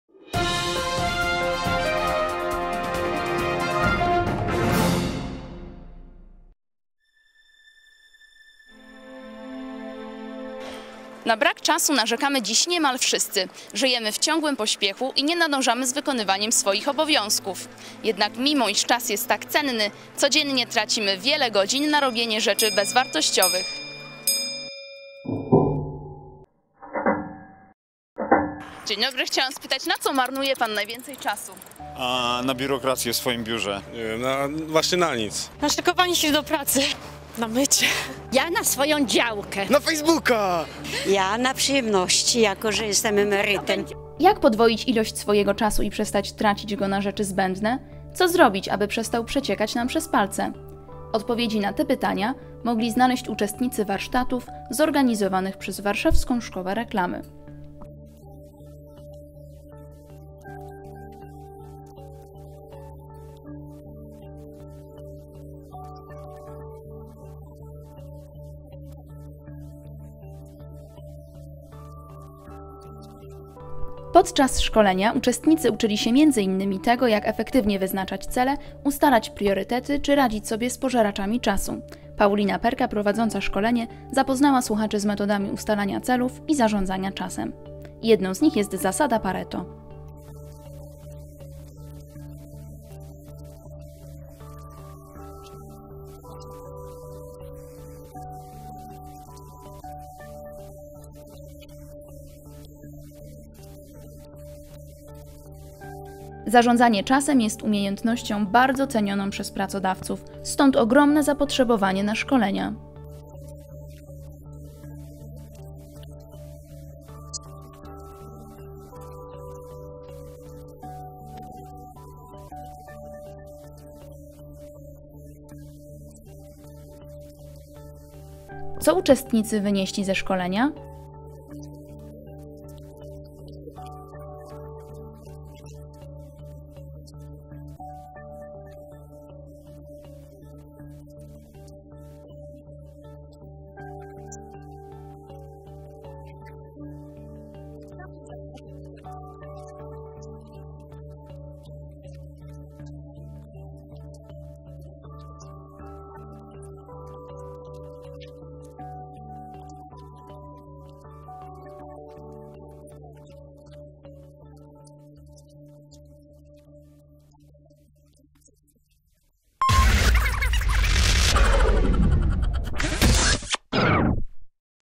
Zarządzanie czasem
W Warszawskiej Szkole Reklamy odbyło się szkolenie z zarządzania czasem. Zajęcia, które przeprowadziła Paulina Perka miały charakter praktyczny. Każdy z uczestników mógł przećwiczyć narzędzia, które pomagają zarządzać czasem . Szkolenie było bezpłatne i miało charakter otwarty.